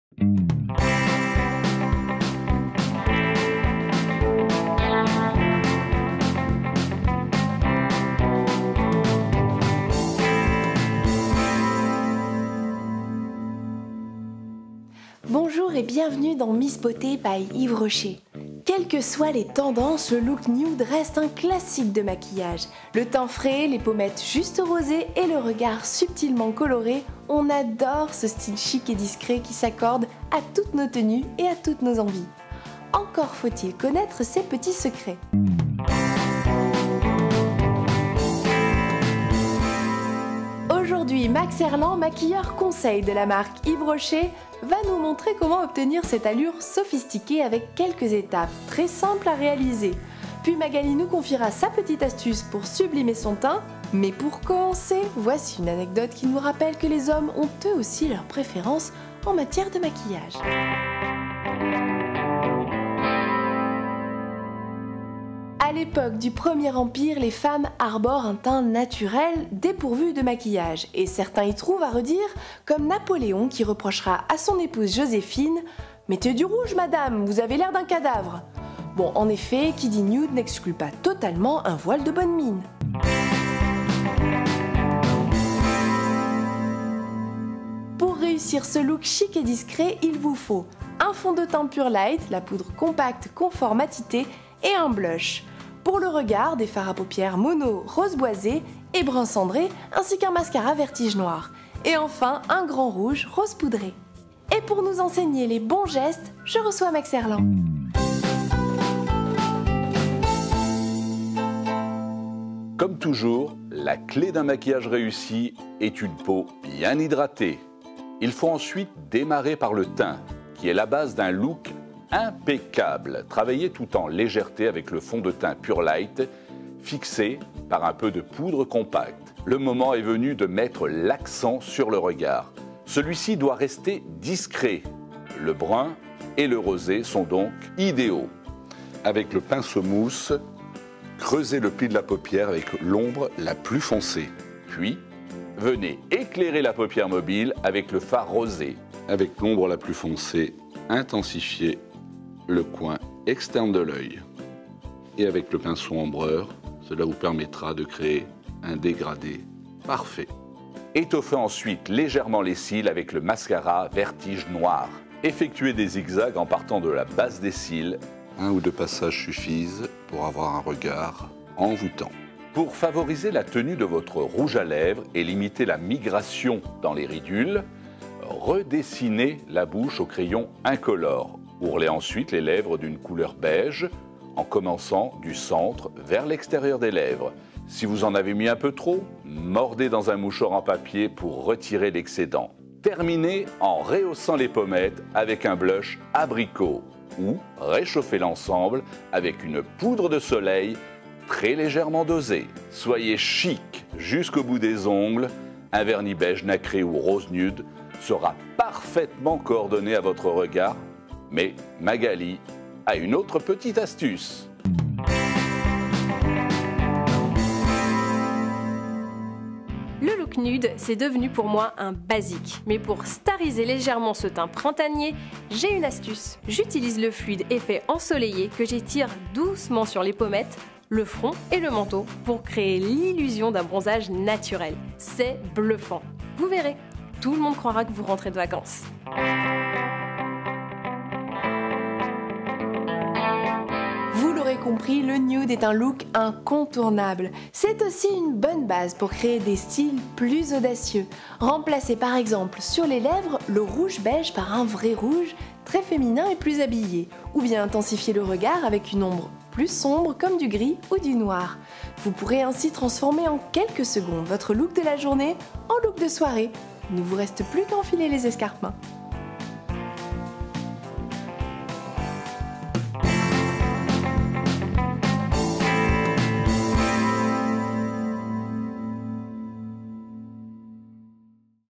Make-up Days Yves Rocher: Přirozený look
"Abychom vytvořili tento chic a diskrétní look, potřebujeme Rozjasňující make-up Pure light, Sypký pudr Couleurs Nature a Kompaktní zmatňující pudr. K líčení očí potřebujeme Mono pudrové oční stíny v odstínu
Rose boisé matné, Brun cendré matné a černou Extra tvarující řasenku. Nakonec použijeme Bezbarvou tužku na rty Couleurs Nature a Rtěnku Grand rouge v odstínu Beige radieux. A jak vše správně aplikovat nám ukáže Max Herlant. "
"Pro dokonalý výsledek je potřeba se zaměřit na bezchybnou pleť. Zkrášlete svou pleť make-upem Pure Light Couleurs Nature. Pro co nejprůsvitnější efekt doporučujeme nejprve použít štětec a aplikaci dokončit houbičkou. Díky této technice výsledné líčení nebude vypadat jak maska.
Pro delší výdrž líčení make-up lehce přepudrujte sypkým pudrem."
"Zaměřme se na hlavní trumf ve svádění: oči. Pro intenzivnější nalíčení použijte tužku hnědého odstínu, kterou za pomoci štětce rozmažete."
"Zvolte klasický způsob líčení, který spočívá v použití temnějšího barevného odstínu (Taupe rosé) pro vnější koutek oka.
Vnitřní koutek oka a pohyblivé víčko rozjasněte odstínem rosé. "
"Pro zahuštění řas naneste cik cak pohybem Extra tvarující řasenku v odstínu Noir. Díky jejímu speciálnímu kartáčku řasy získají maximální objem již po nanesení první vrstvy.
Nakonec rty zvýrazněte trochou Lesku na rty Sexy Pulp v odstínu Caramel, který Vašim rtům dodá svěží nádech. "
"Tvářenku nanášejte plochou stranou štětce. Rozetřete ji směrem ke krajům obličeje. Pokud si nejste jisti, kam přesně tvářenku nanést, nejprve se usmějte a tvářenku pak naneste na vystouplé části tváře. Pro zářivý vzhled až po konečky prstů nezapomeňte použít Zářivý lak na nehty Couleurs Nature v odstínu Rose nude. Naneste tenkou vrstvu, začněte od středu nehtu.
Ale Magali má pro nás ještě další radu. "
Tento přirozený styl líčení jsem si naprosto oblíbila! Abych docílila hvězdného vzhledu, mám pro Vás malý trik: používám Slunečný pudr, který dodá líčení efekt přirozeného opálení. Slunečný pudr naneste ke krajům obličeje. Je to dobrý trik, díky kterému si všichni budou myslet, že jste se právě vrátila z dovolené!
"Jak jste již asi pochopili, přirozené líčení je základní look. Je to také dobré základ pro výraznější líčení, stačí nahradit Rtěnka Grand rouge v odstínu Beige radieux velmi ženským červeným odstínem. Nebo zvýrazněte svůj pohled tmavším odstínem Mono pudrových očních stínů, jako je šedivá nebo černá, takto můžete proměnit za pár sekund Vaše denní líčení na večerní! Zbývá už jen si nazout podpatky. "